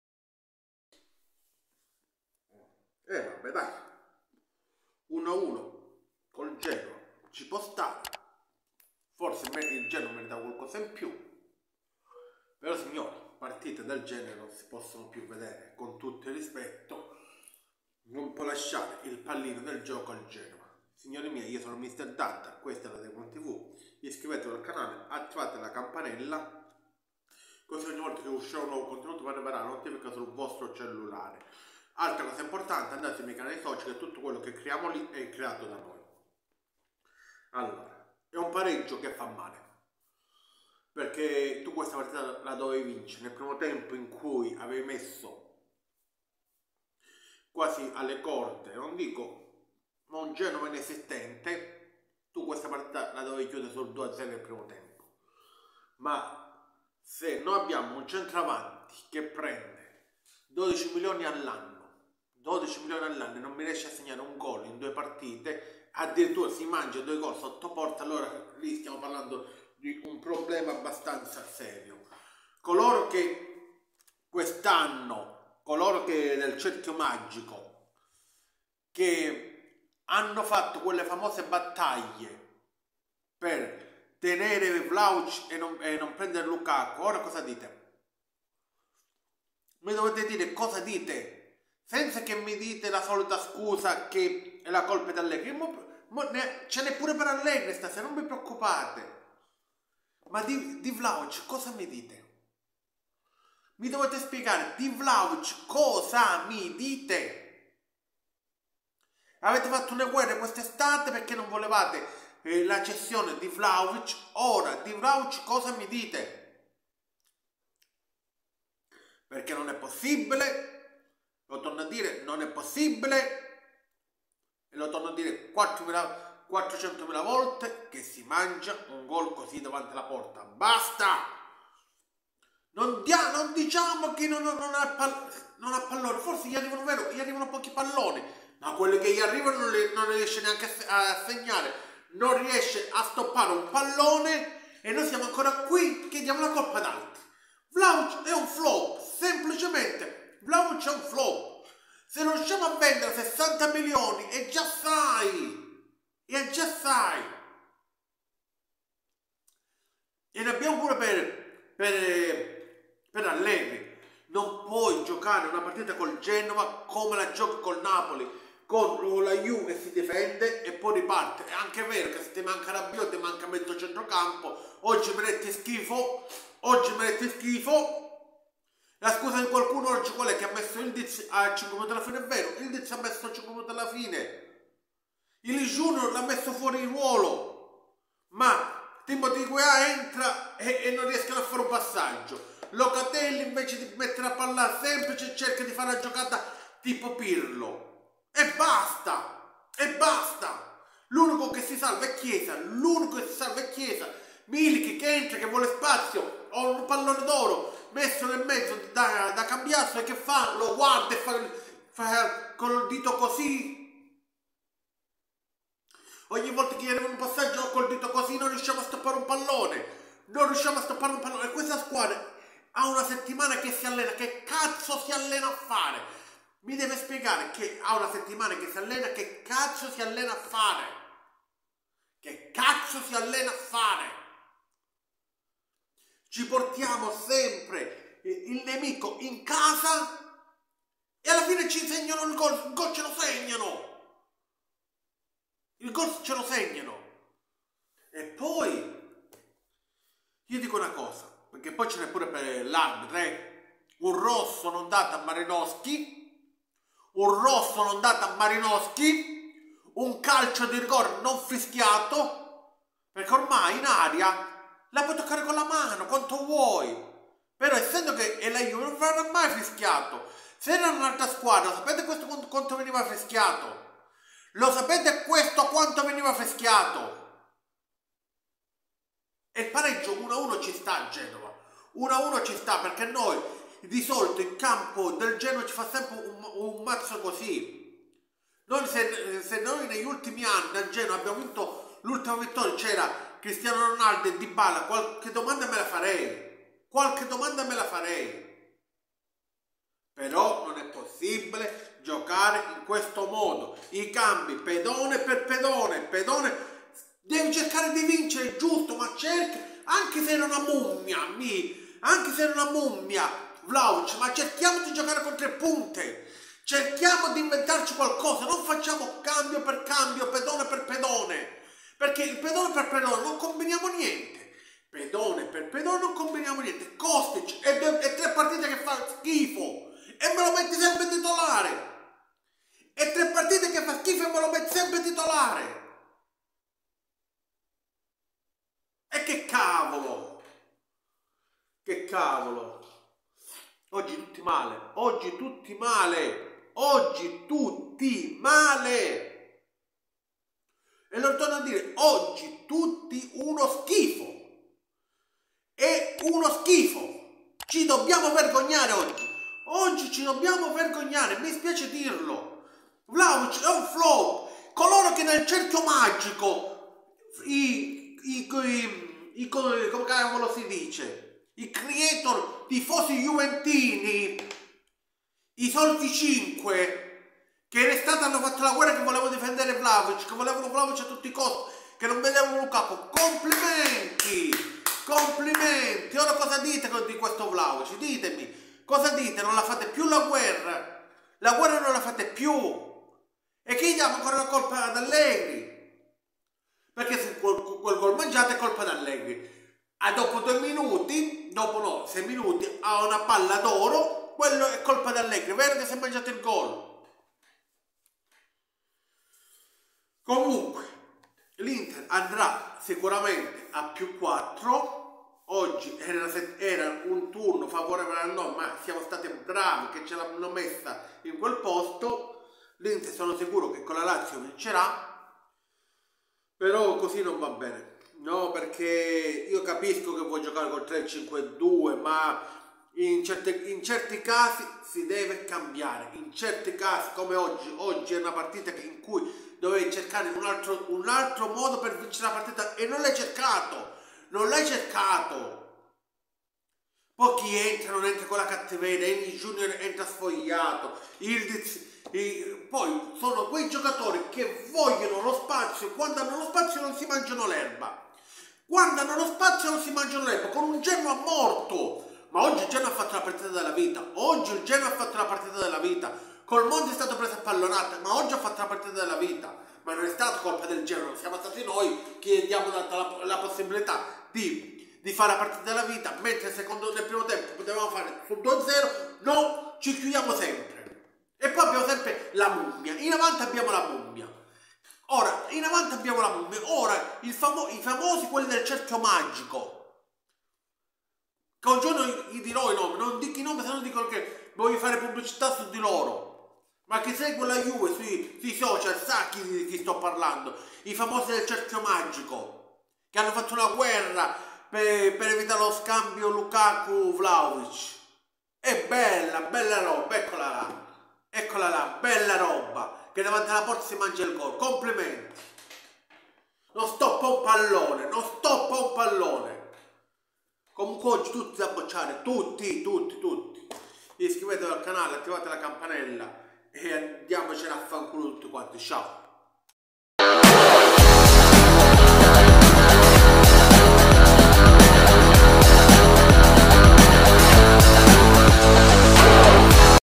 😱Così non va,ci vuole un bagno di umiltà.🤬 Ora Vlahovic diventa un caso😵
l Genoa ferma sull'1-1 a Marassi la Juventus, che resta così -1 dalla capolista